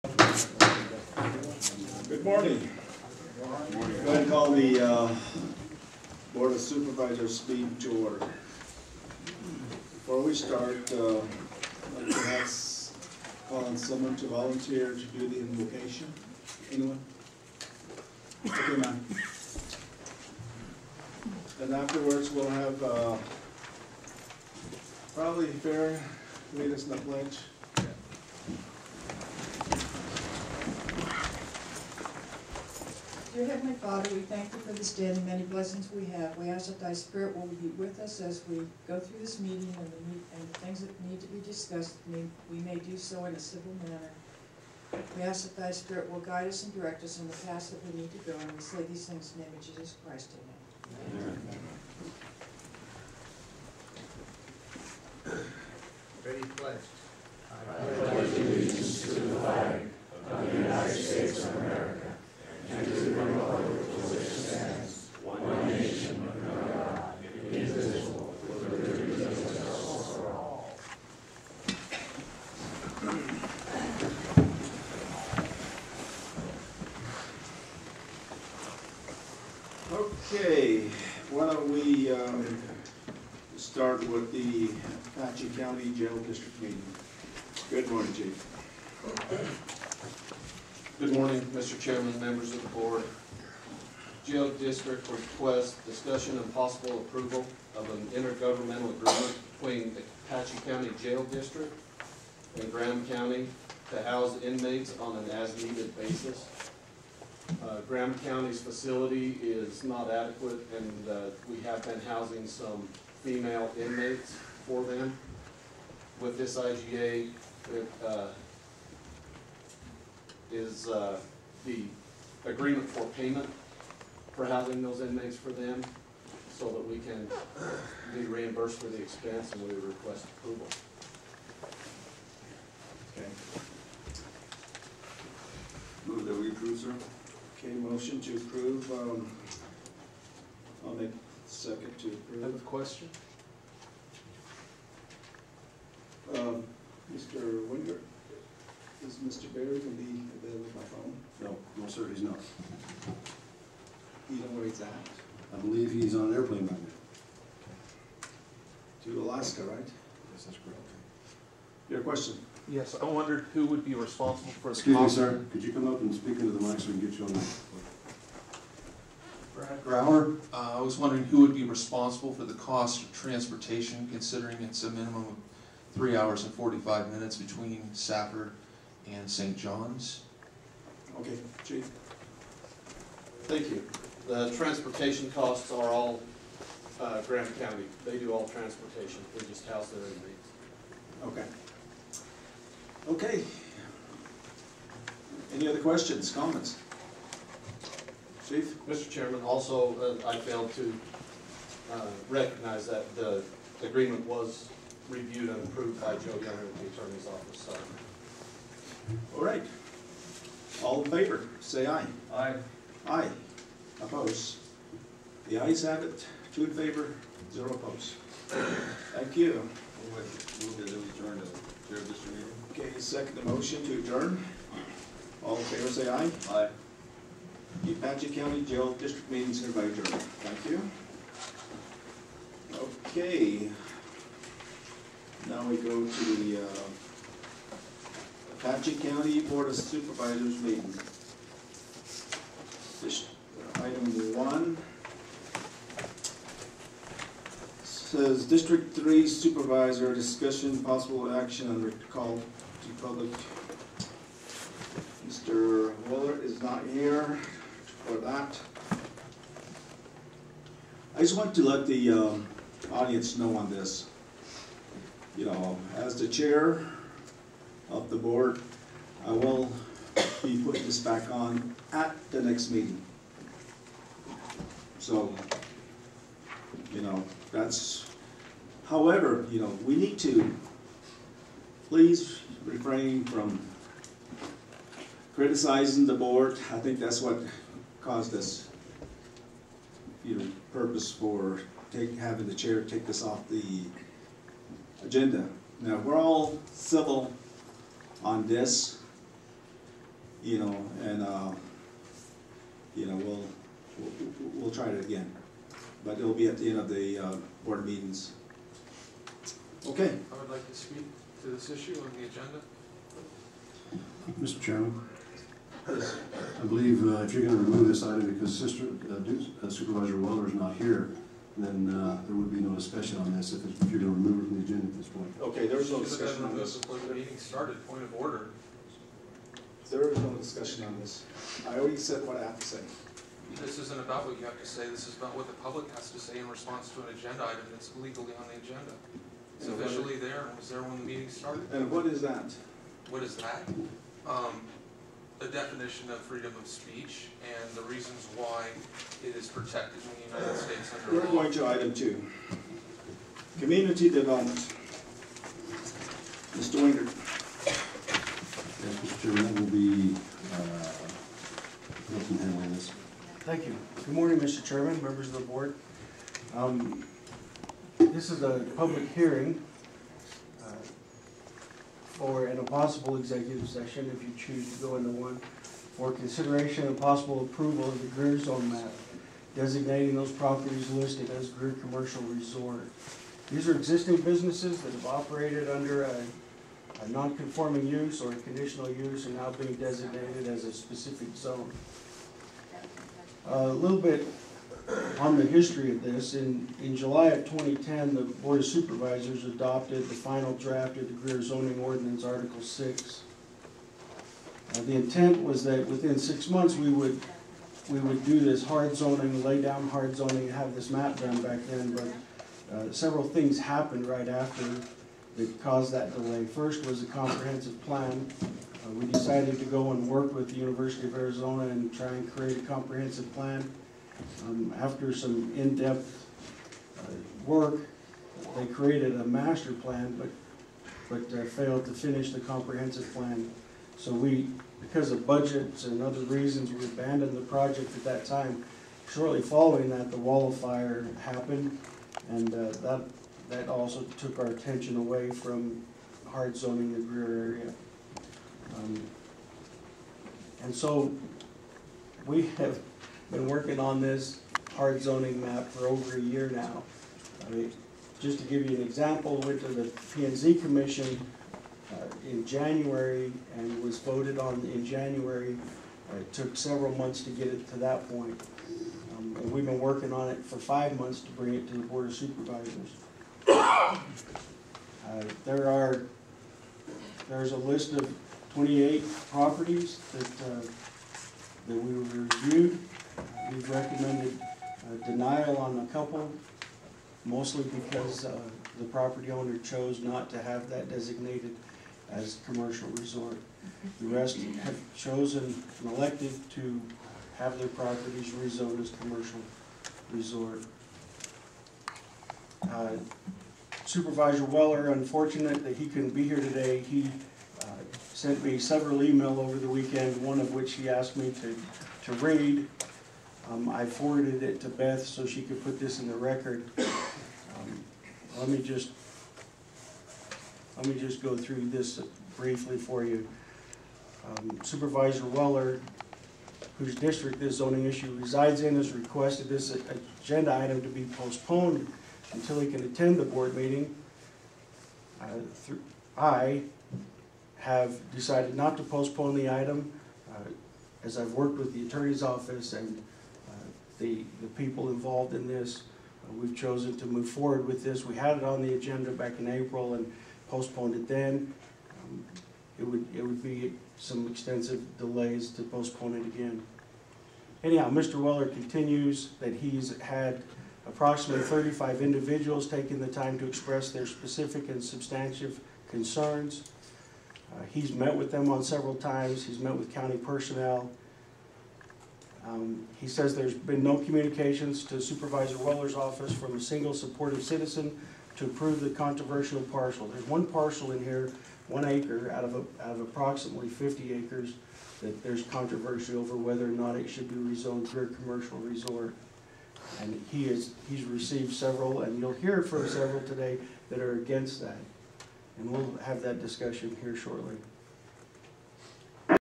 May 19, 2014: Apache County Board of Superviosors Meeting- Part One
Part one of the Apache County Board of Supervisors meeting held in St. Johns, Arizona on May 19, 2014.